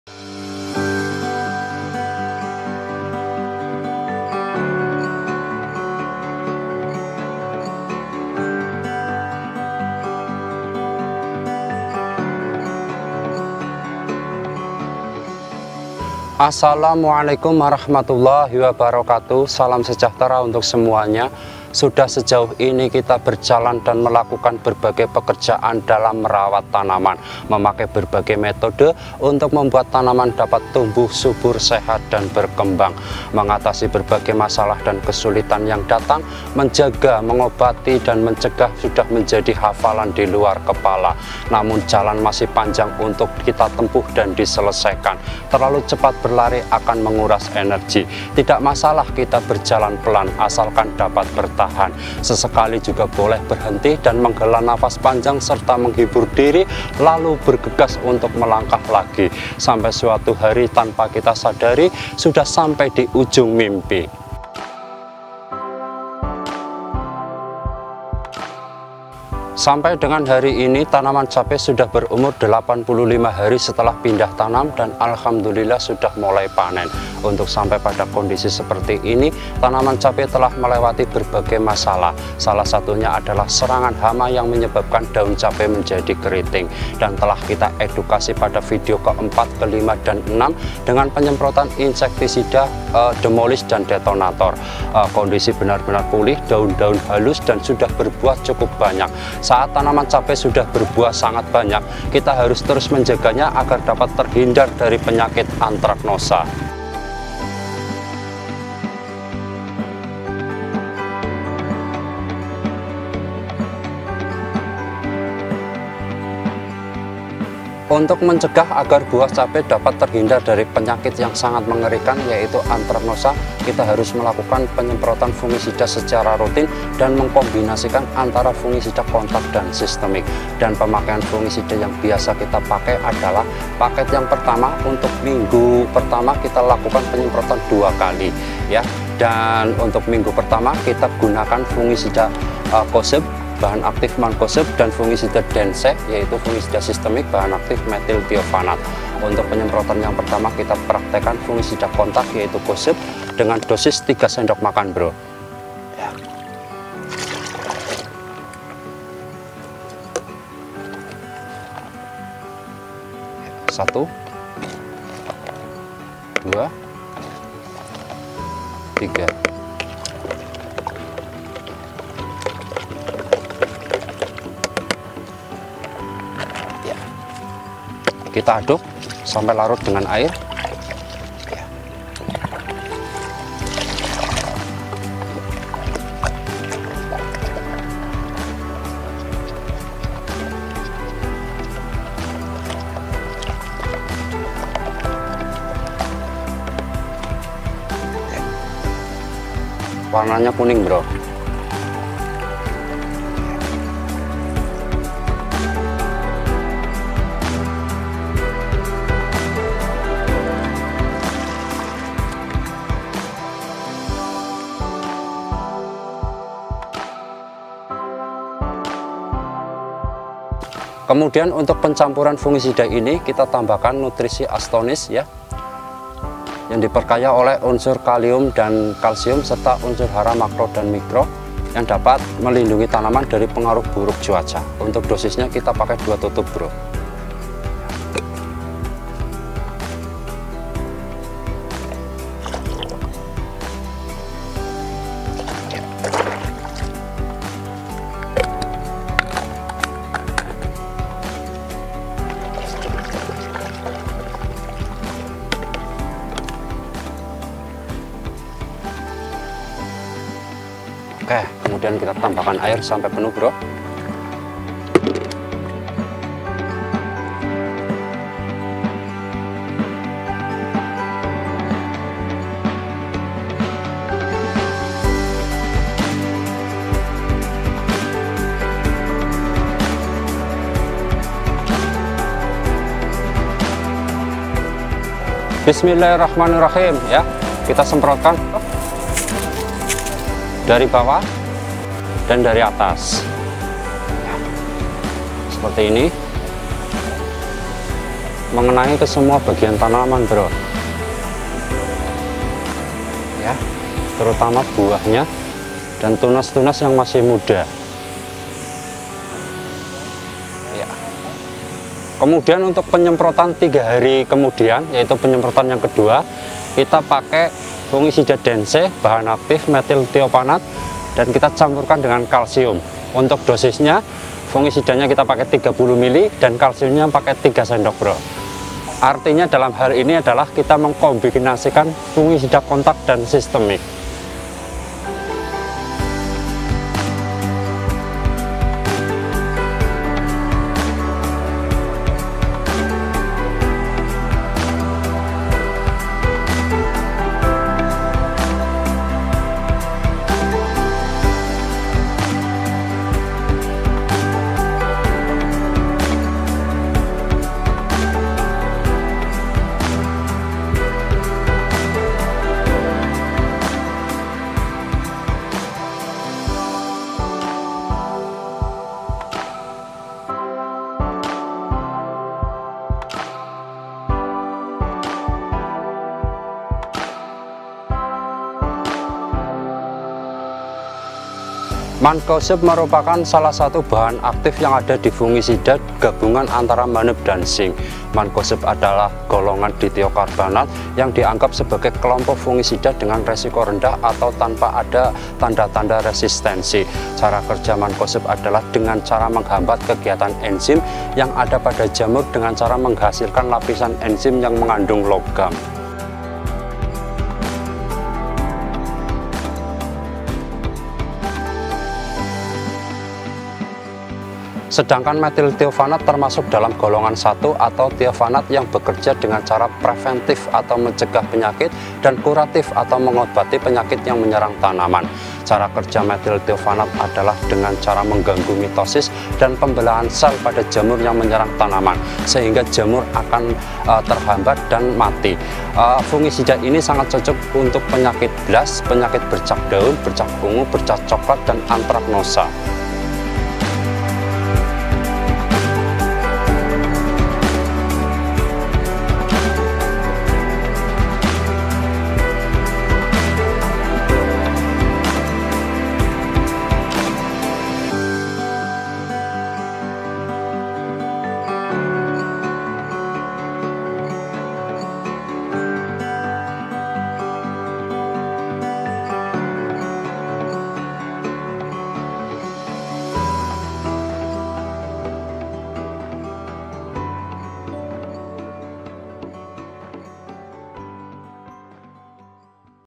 PAKET FUNGISIDA #1 UNTUK MENCEGAH ANTRAKNOSA
Untuk dapat mencapai kesuksesan dalam budidaya tanaman cabai, segala usaha mesti di lakukan untuk membuat tanaman cabai sehat, subur dan berbuah banyak. Kemudian yang kedua buah cabai harus terhindar dari segala penyakit khususnya antraknosa.
Penyakit ini sangat mengerikan dan dapat menyebabkan kerugian besar karena perkembangannya sangat cepat.
Video kali ini akan mengedukasi cara mencegah penyakit antraknosa dengan penyem[rotan fungisida secara rutin dan memakai metode khusus.

Semoga Bermanfaat.
Semua Tempat adalah Sekolah
Semua Orang adalah guru

Semoga Sukses Selalu
SUDAH SAATNYA PETANI SEJAHTERA.